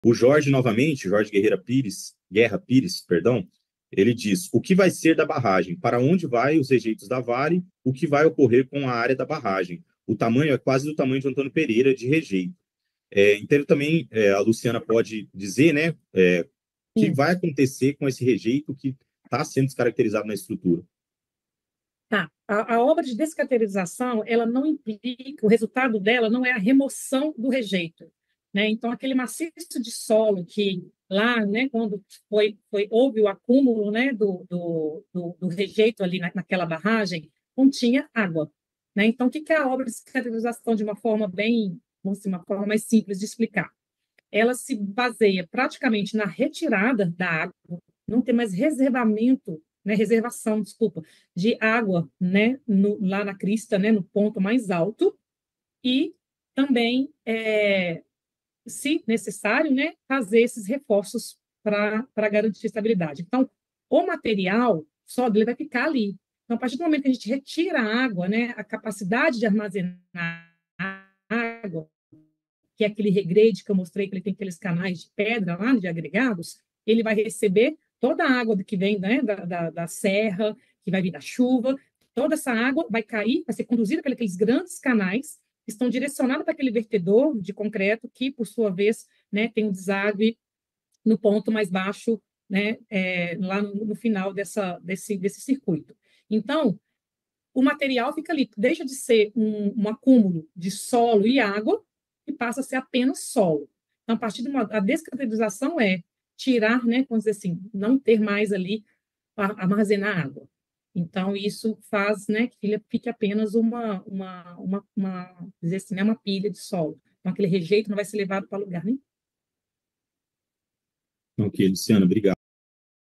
Perguntas e Respostas sobre a barragem doutor: o que vai ocorre com o rejeito na barragem doutor?
Perguntas e Respostas sobre a barragem doutor: encontro online com a Vale 17/10/2024. (Antônio Pereira, Ouro Preto)

Vou tentar transmitir no dia, se tiver problemas, fonte original

Estou usando StreamYard, excelente para fazer lives. Super-fácil, sem muita preparação.
Link abaixo para usar. Ao usar a versão paga, eu ganho créditos!